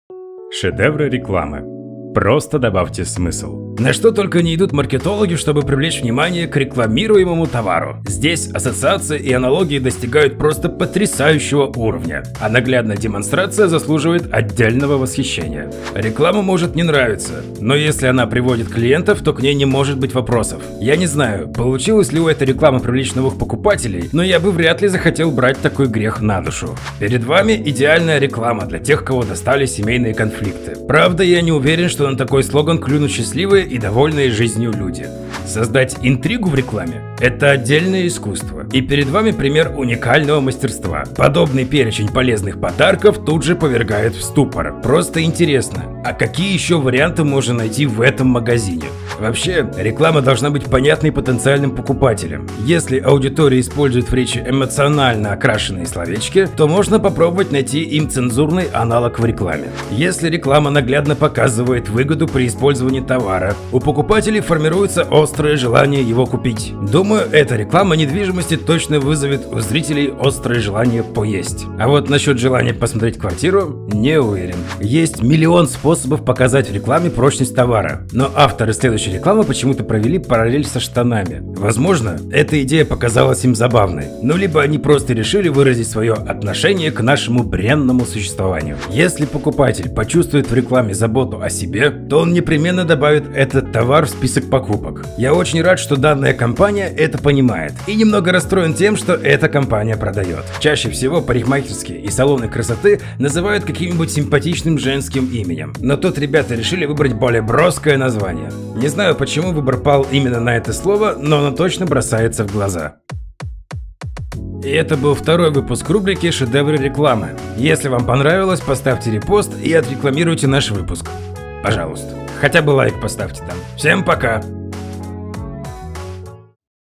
«Шедевры» русской рекламы. Выпуск 2
«Шедевры» русской рекламы — рубрика, в которой я смешно обозреваю «шедевры» русской рекламы. Большой выпуск 2

С любовью Zinoink ;)